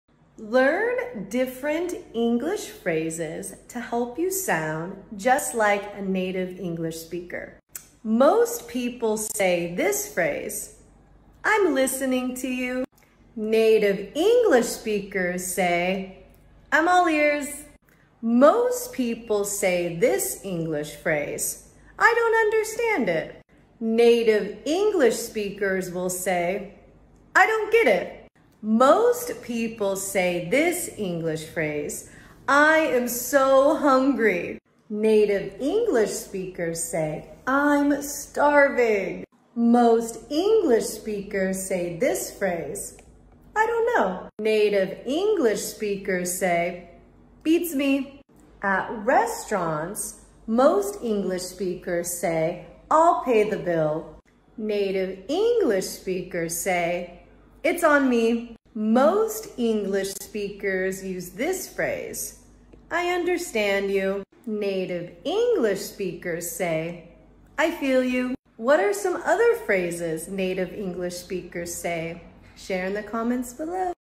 Speak English Like a Native 🇺🇸 Learn Different Phrases Native English Speakers Say
Check out this Video to help you Speak English like a Native 🇺🇸

Learn English Phrases: Different Phrases that Native English Speakers Say

Need more help learning English? 😁

Join my Live Zoom Lesson!

Follow and Like My FB Page 👇🏻 Join Live Zoom Lessons